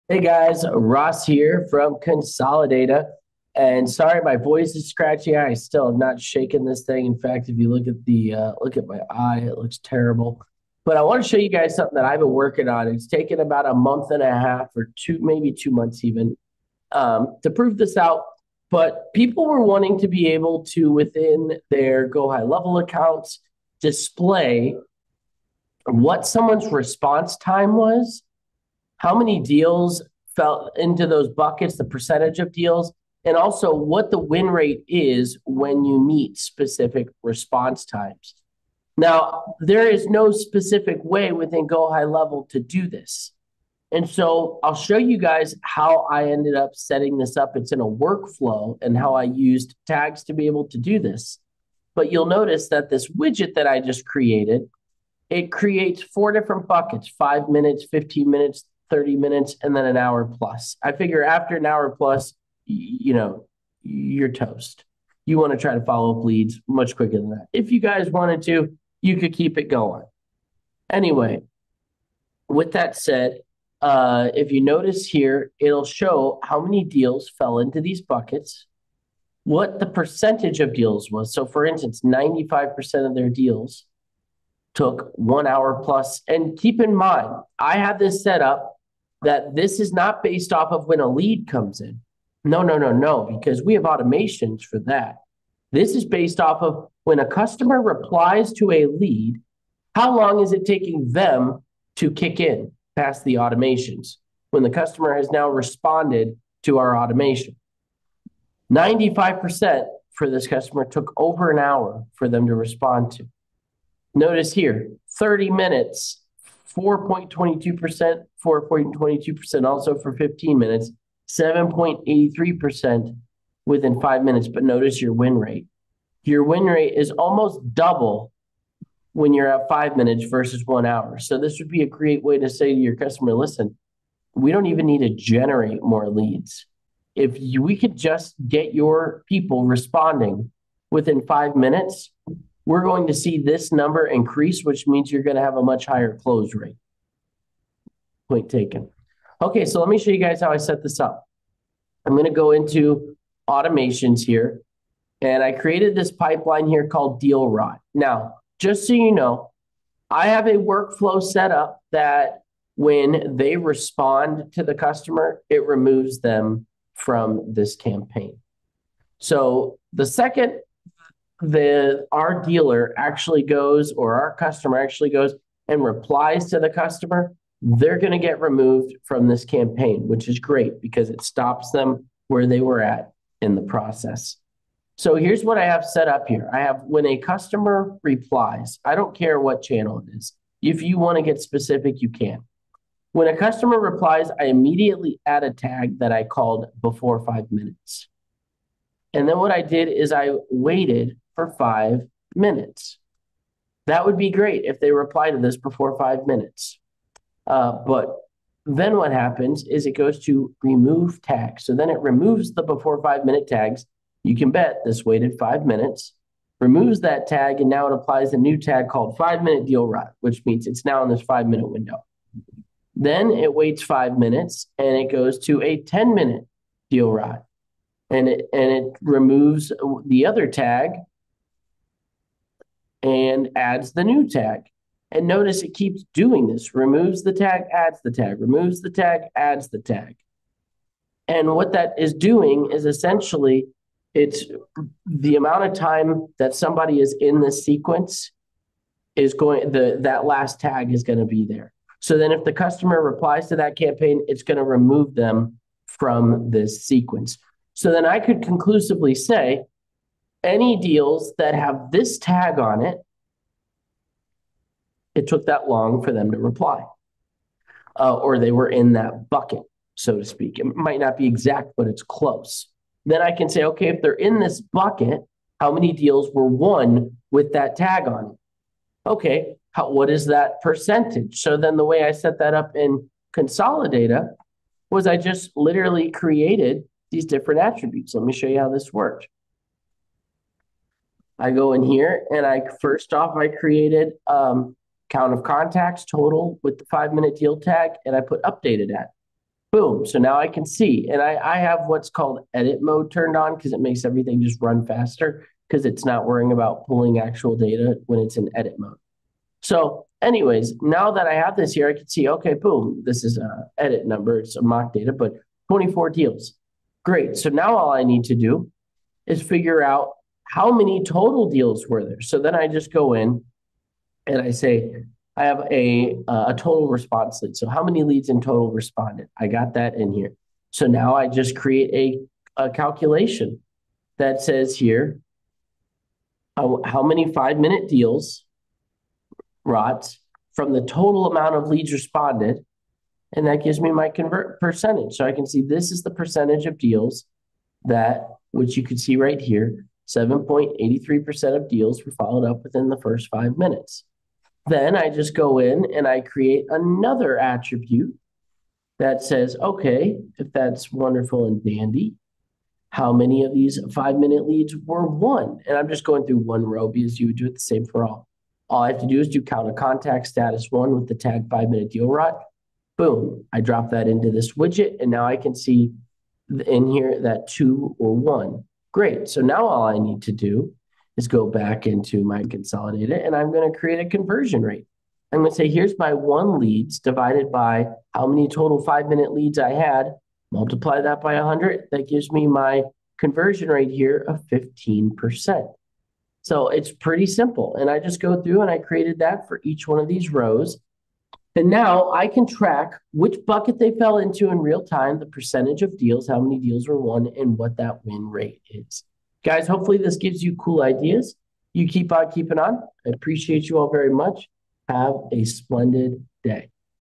How To Track Response Times With Consolidata & HighLevel
Are you looking to improve your customer service and ensure timely responses to your clients? Look no further than this tutorial on how to track response time in GoHighLevel CRM using the Consolidata reporting tool.

In this step-by-step video guide, we'll show you how to use the powerful Consolidata reporting tool to track your response time in GoHighLevel CRM. You'll learn how to set up the reporting tool, configure the necessary parameters, and generate reports that provide valuable insights into your response times.

By tracking your response time, you'll be able to identify areas where your team can improve their customer service, as well as areas where you're excelling. This will allow you to adjust your approach and make data-driven decisions to optimize your customer service strategy.

Whether you're a small business owner looking to improve your customer service or a large enterprise looking to track and optimize response times across multiple teams, this video has something for you. So sit back, grab a cup of coffee, and let us show you how to track your response time in GoHighLevel CRM using the Consolidata reporting tool.To get started with